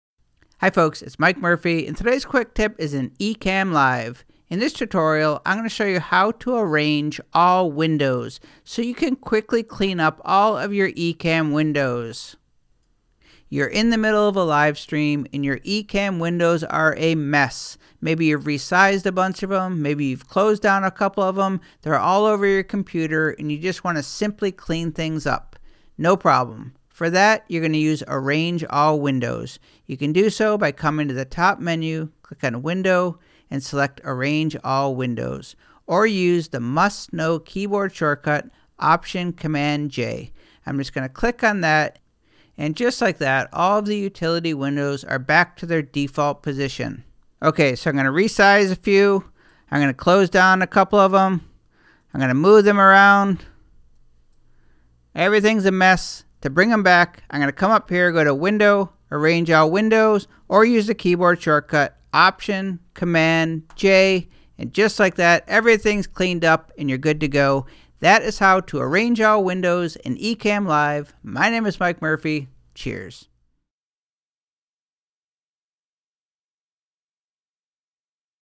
Ecamm Live: How To Arrange All Windows
This tutorial will show you how to clean up your Ecamm Live workspace by using ‘Arrange All Windows’ which will show all Utility Windows (except the Network Camera) and place them in the default position.

Why use Arrange All Windows?
It is a fast way to ‘reset’ and clean up your Ecamm Live production environment so you can improve your Livestream experience by keeping your production clean and organized.

Keyboard Shortcut:Option + Command + J

How To Arrange All Windows in Ecamm Live
The easy way is to use the keyboard shortcut Option + Command + J or,
Go to the top menu and click on ‘Windows’
Click on ‘Arrange All Windows’
Utility Windows will be arranged neatly in their default position
All default utility windows will be visible even if closed them previously